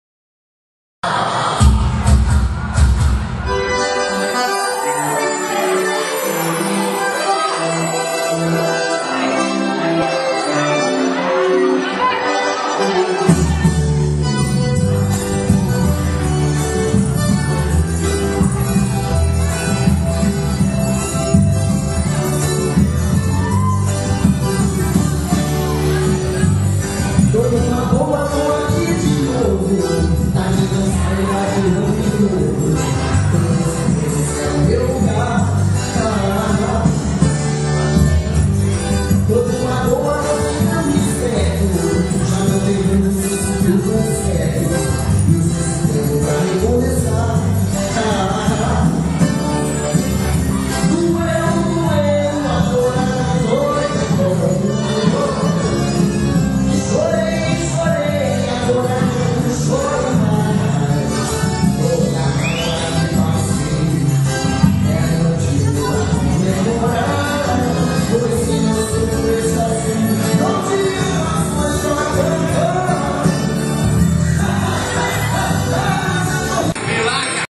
preferencia musical em estilos - forro 1 da epoca da festa
Orquestra a Quatro Mãos festa com DJ momento estílo forró da época da festa  referência na escolha dos estílos musicais na ficha do DJ.